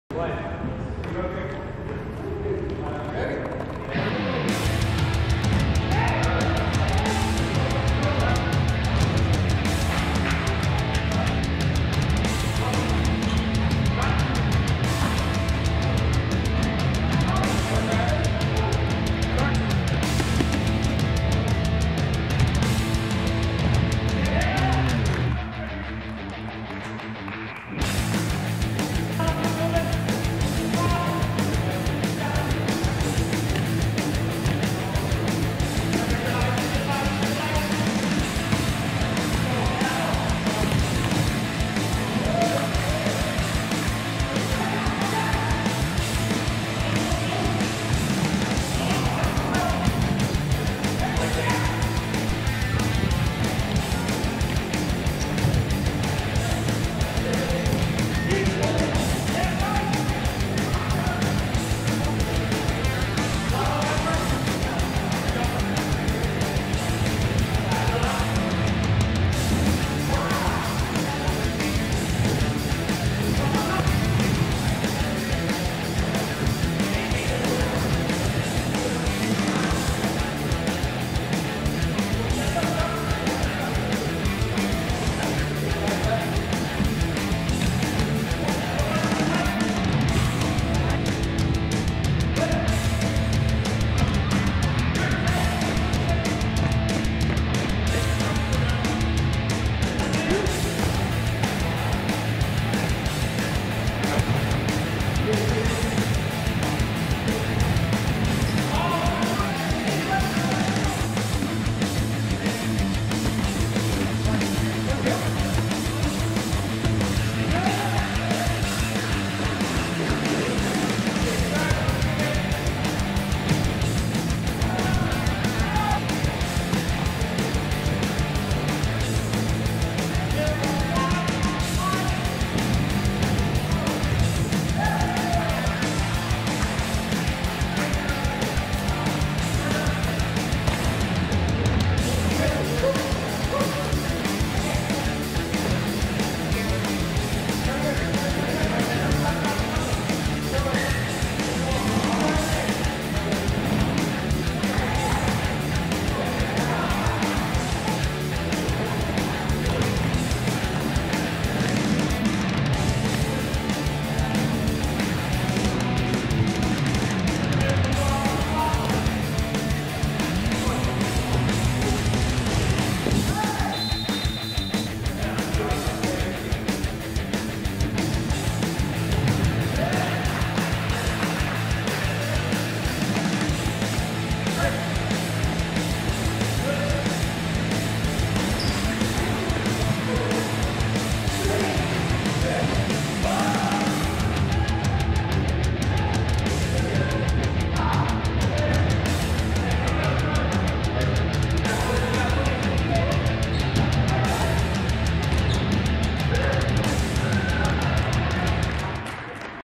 Cincinnati Futsal - 12-11-2019 Part 1
Enjoy the first part of highlights from our Futsal opening night!